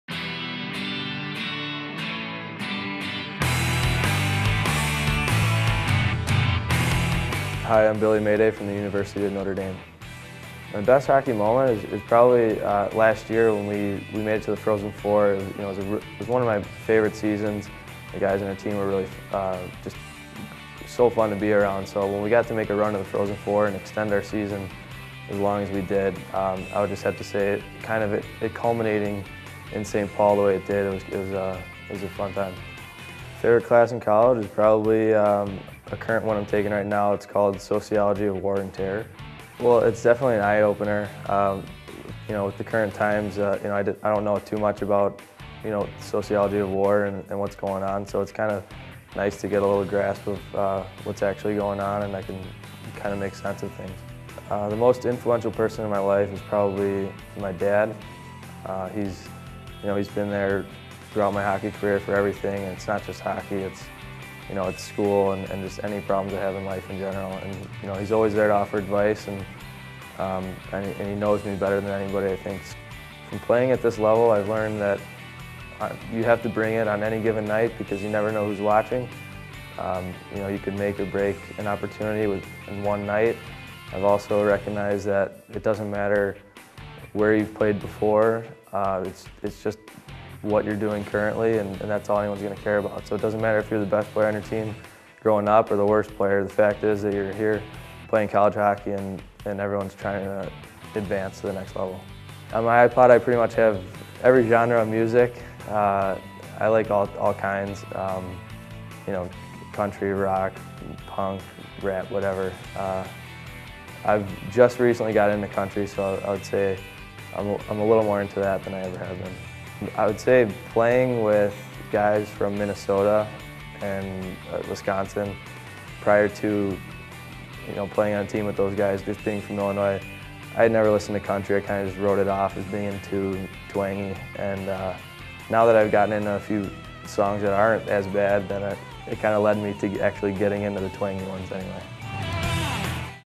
20 Questions with Billy Maday
Billy Maday talks about going to the 2011 Frozen Four, "bringing it" on any given night, and his new-found love for country music.